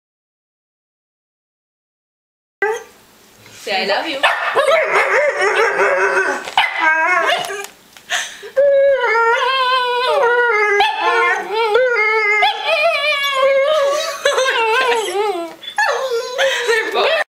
Lhasa Apso Mimics Her Pit Bull Mix Sister
They really want to have the Port Wine Cheese I have and seem to have teamed up on me to get it! Normally my Lhasa just barks, but this time is different. Notice at the begining she starts by barking (typical), but quickly changes her bark to mimic her sister Jade.